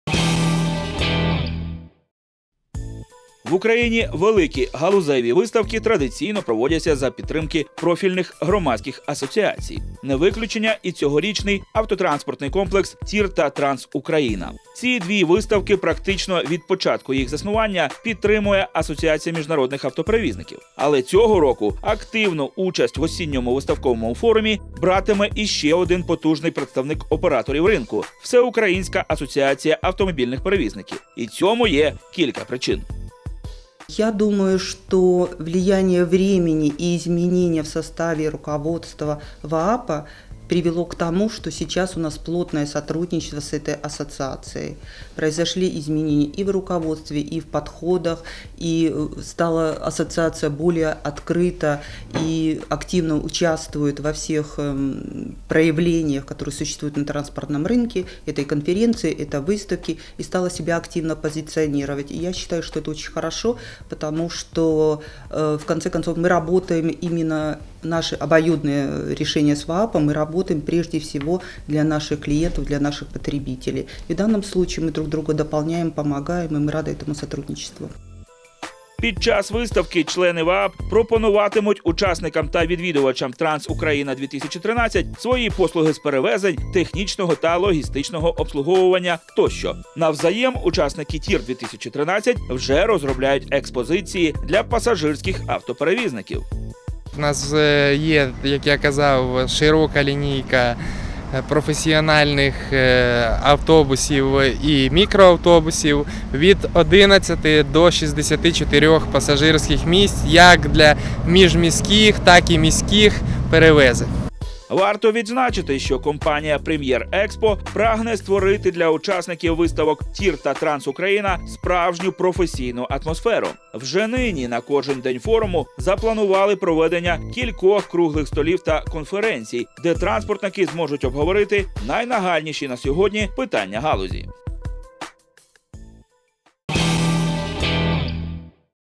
TIR&TransUkraine 2013 - videodiary by Fbc.net.ua
За 2,5 месяца до старта транспортных выставок TIR'2013 TransUkraine'2013 на Первом деловом телеканале начал выходить цикл сюжетов в специальном проекте "Транспорт". Об отрасли и выставке рассказывают сами участники.

Смотрите все выпуски на этом виделканале или на сайте